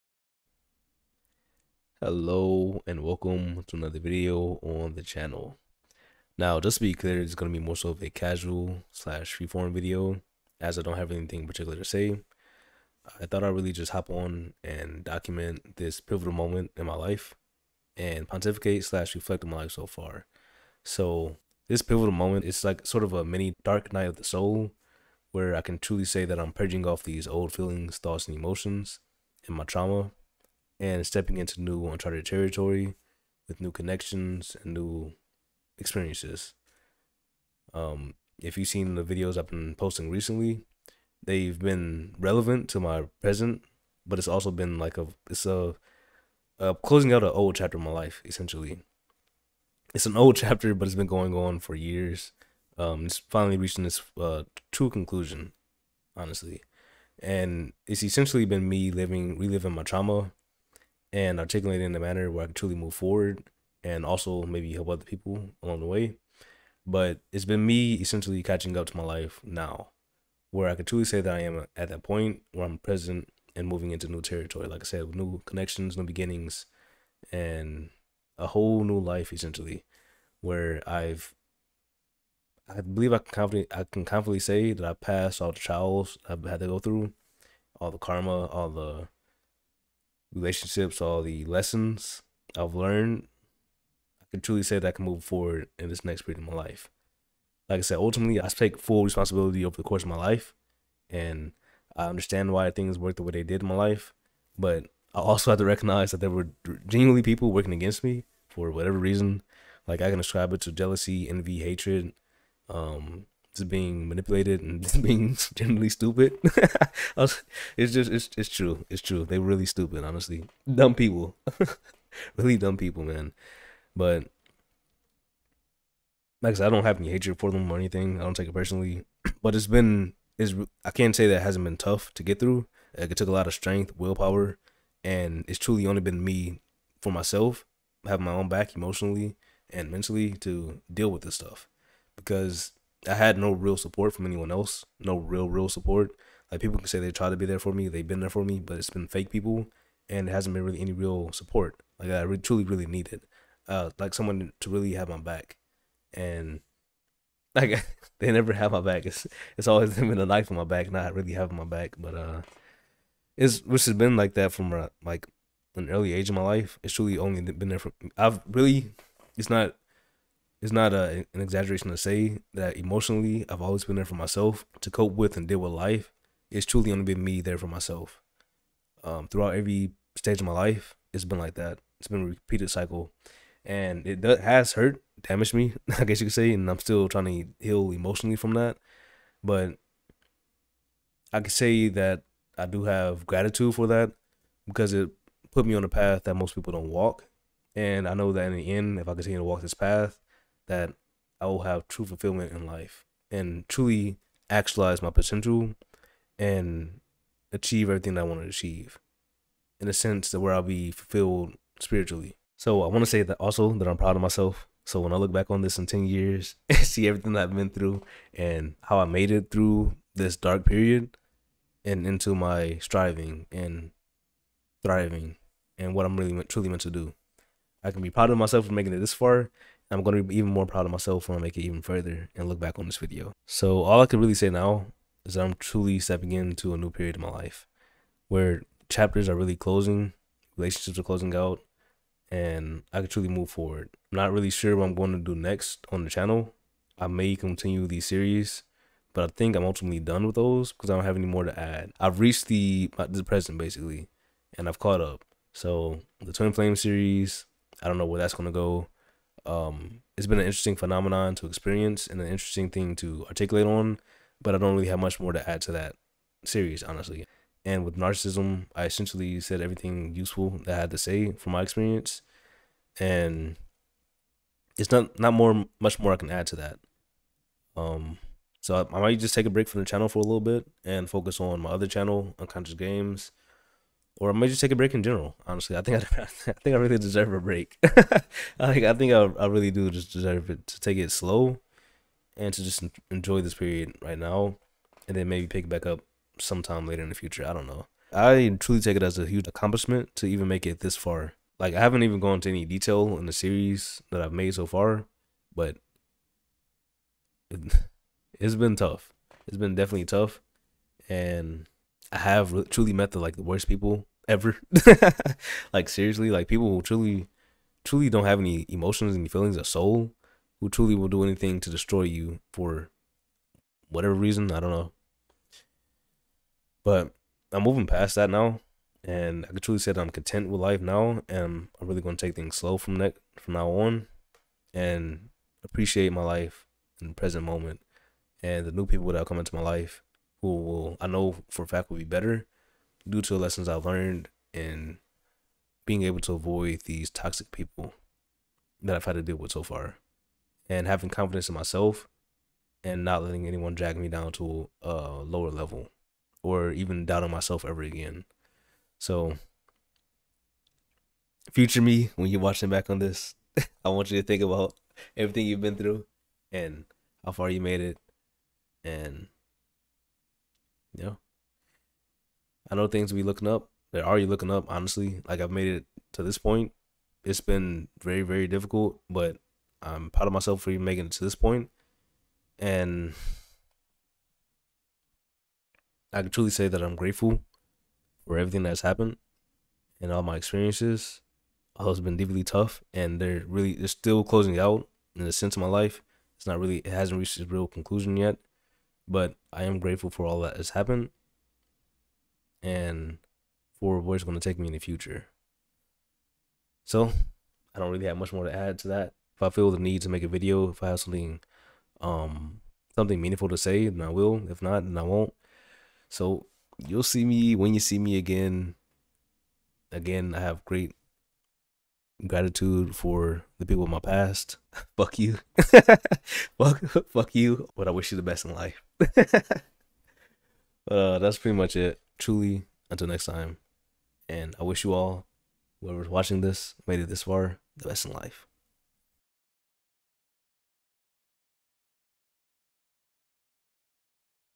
Old Endings, New Beginnings | The End
This video is moreso a note to self where I reflect and pontificate on my life so far where things are reaching their true conclusion in purging old emotions, connections, and lessons and stepping into the next chapter of my life and what's meant for me.